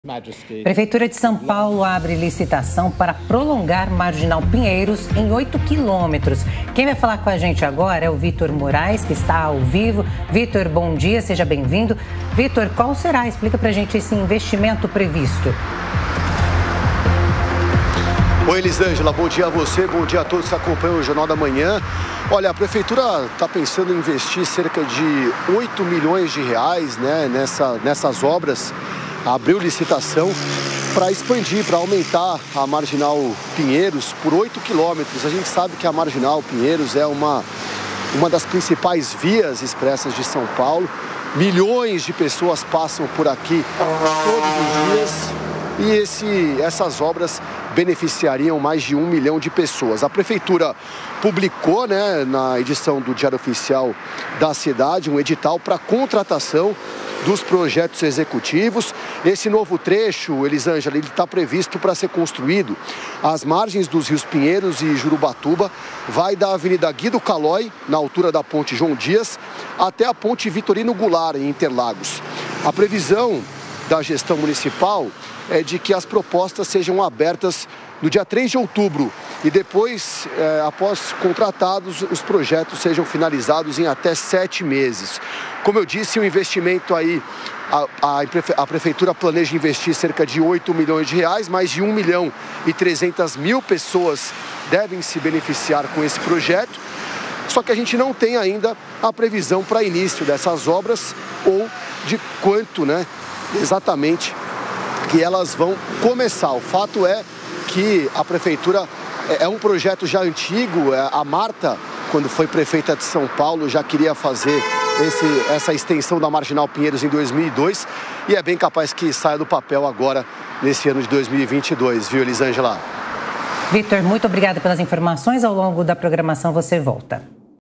Marginal Pinheiros pode ser ampliada em até 8 km
A Marginal Pinheiros pode ser ampliada em até 8 km. A prefeitura da capital abriu licitação neste domingo (11) para investimento de R$8 milhões. Novo trecho será construído no trecho entre o rio Pinheiros e o rio Jurubatuba.